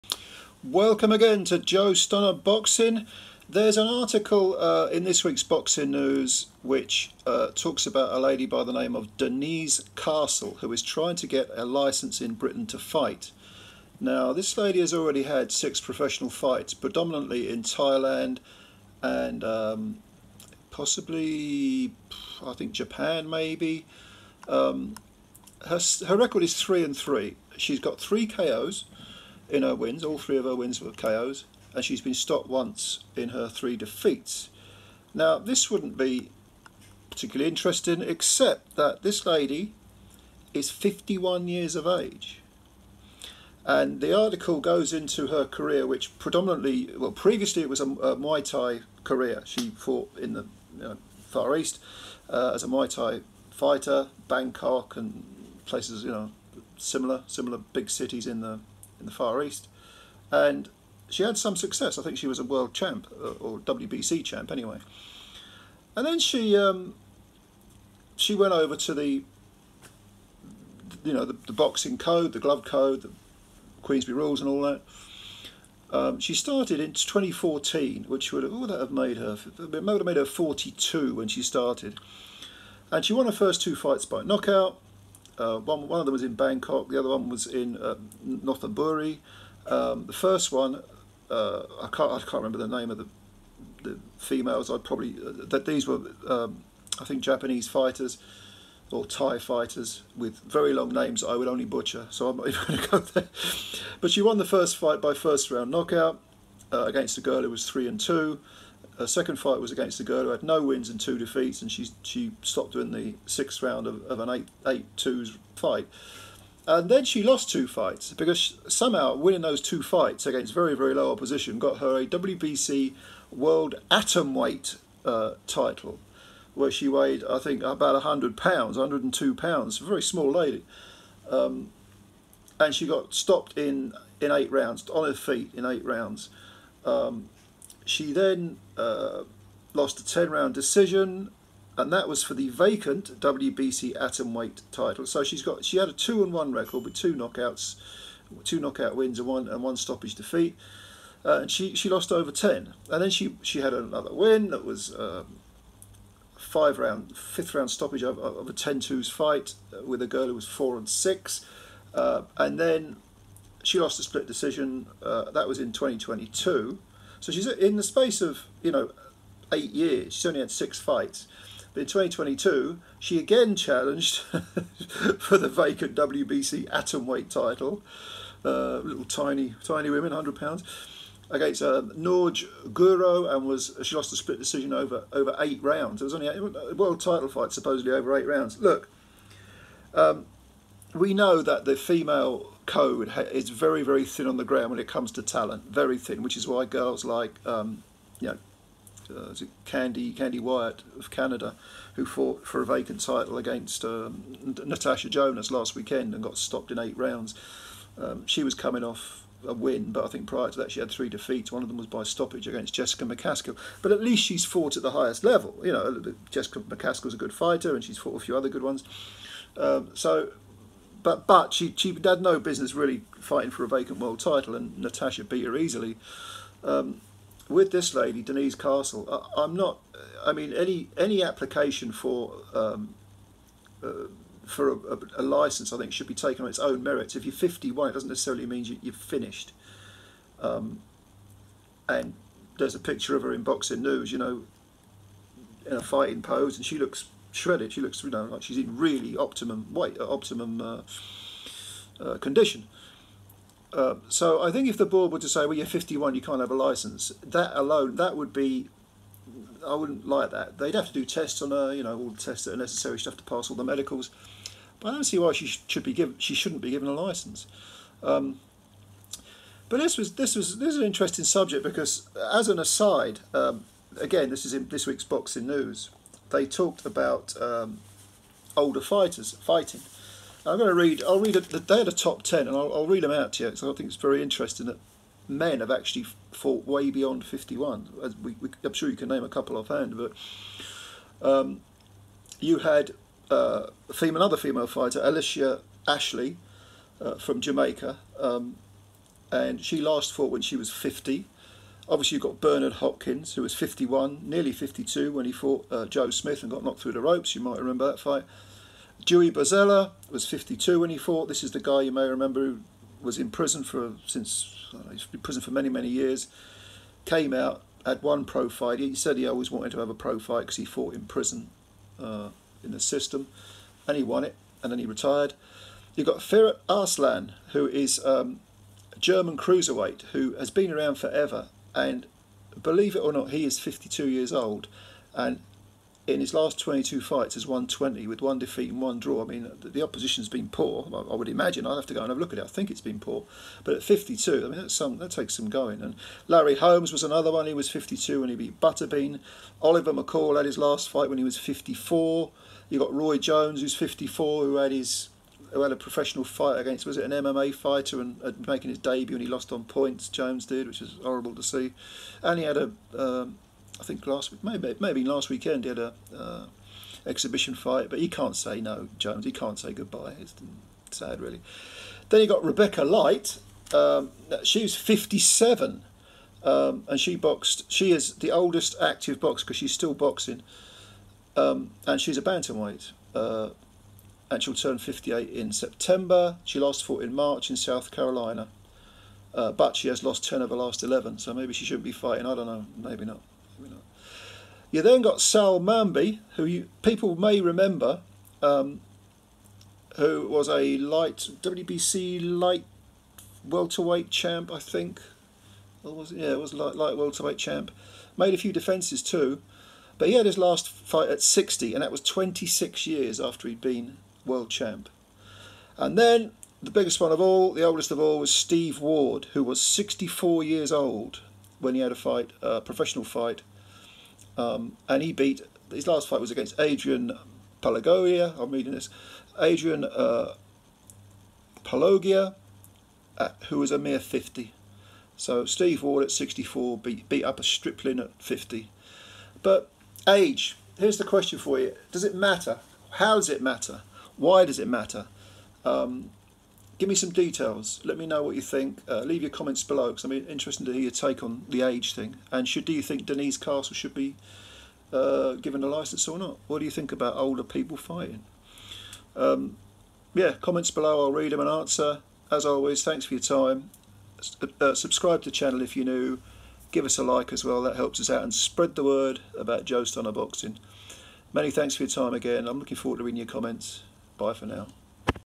When is old too old? DENISE CASTLE, aged 51, applies for a Board licence.
Denise Castle wants to build on her six-fight career with a British Boxing Board of Control licence, despite being 51 years of age.  Would it be ageist to deny her a licence simply when there are examples of other fighters - men and women - who've fought beyond the half-century mark, or should cautious safety always come first?